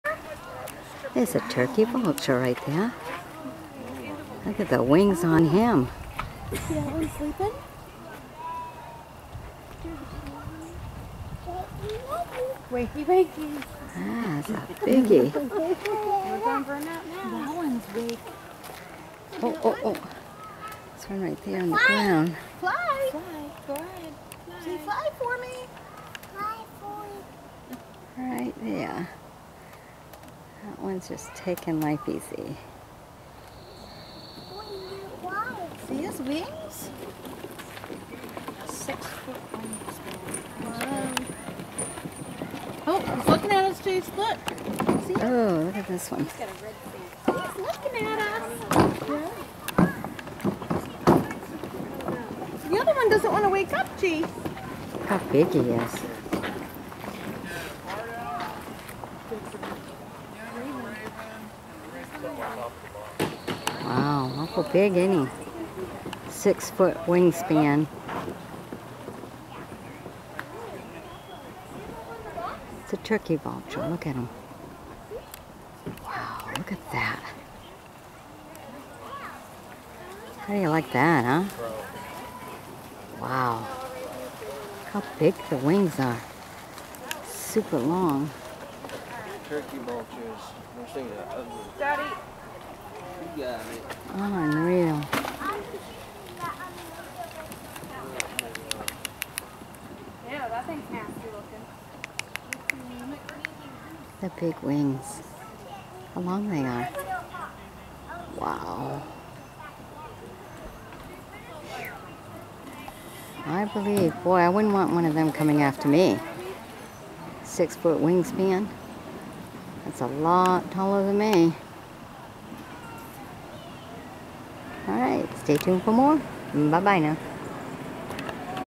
Wingspan Of A Turkey Vulture
Check out this huge turkey vulture and its six foot wingspan.